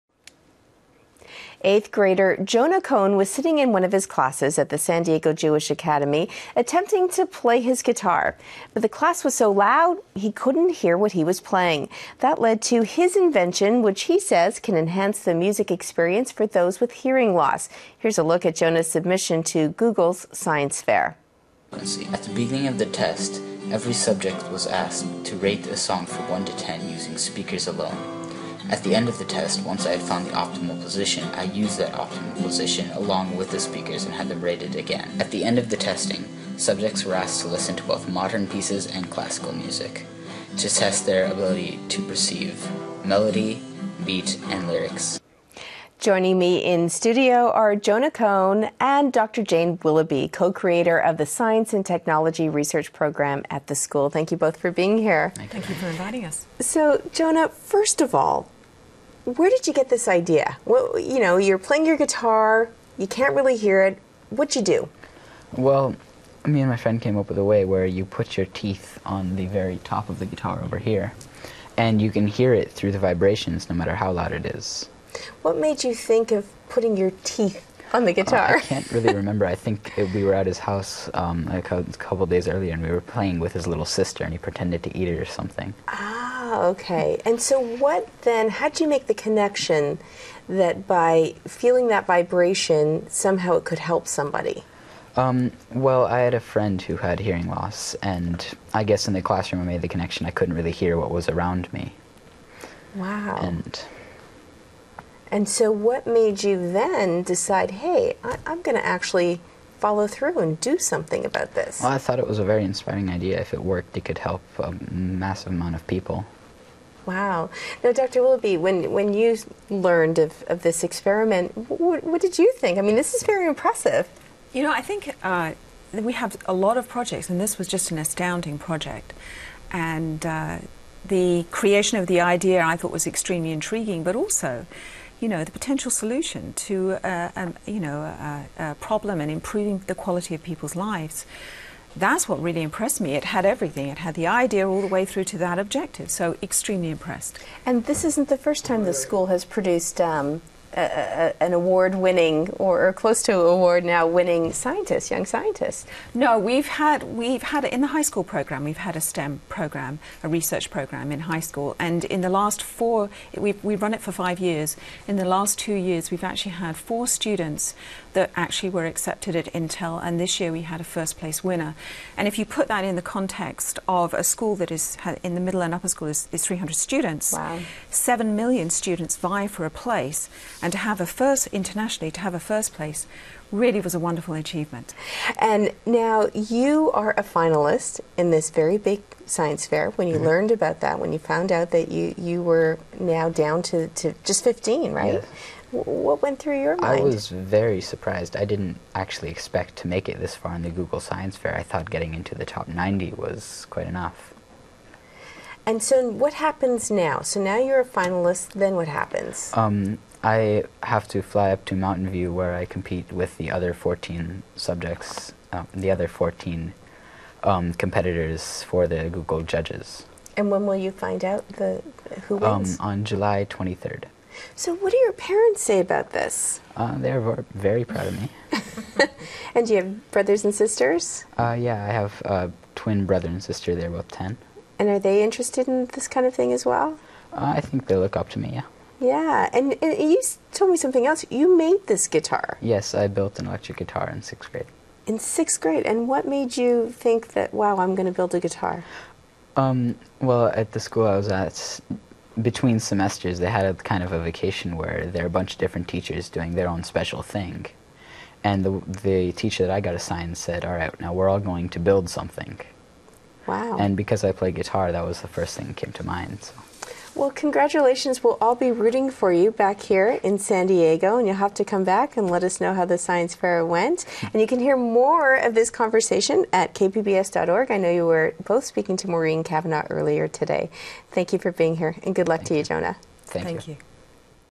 Jonah Kohn, Google Science Fair Competition
Jonah Kohn, a student finalist in the Google Science Fair competition, and Jane Willoughby, a co-founder and creator of the science and technology research program at the San Diego Jewish Academy.